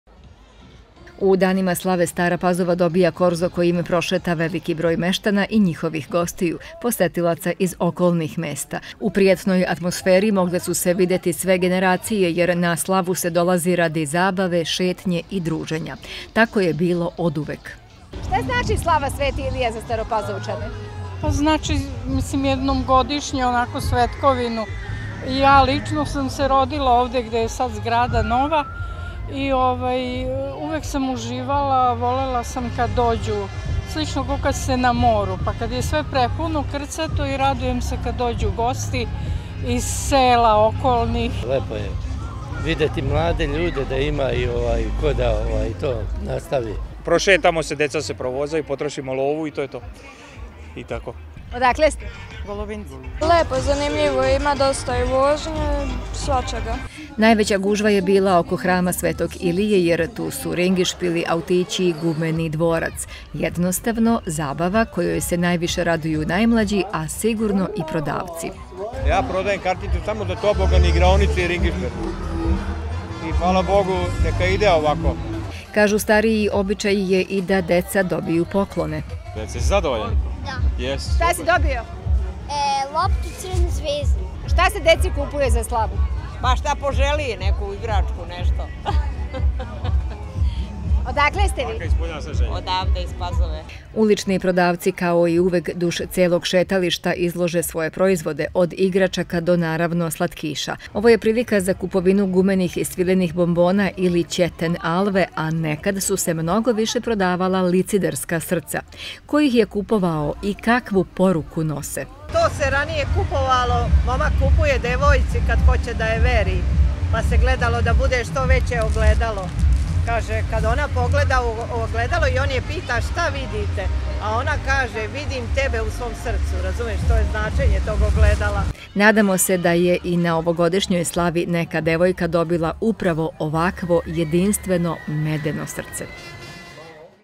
Slava u Staroj Pazovi uz obavezne ringišpile
Trodnevna manifestacija Dani Opštine Stara Pazova koja se ove godine obeležava od 1. do 3. avgusta proistekla je iz višedecenijske slave mesta  -  Sveti Ilija Gromovnik i to na inicijativu predsednika Opštine Đorđa Radinovića pre pet godina. Uz obilje sadržaja, zabavno – muzičkih programa, luna park postavljen oko hrama Svetog Ilija predstavlja tradiciju, kojoj se svi raduju a tako je bilo i ove godine.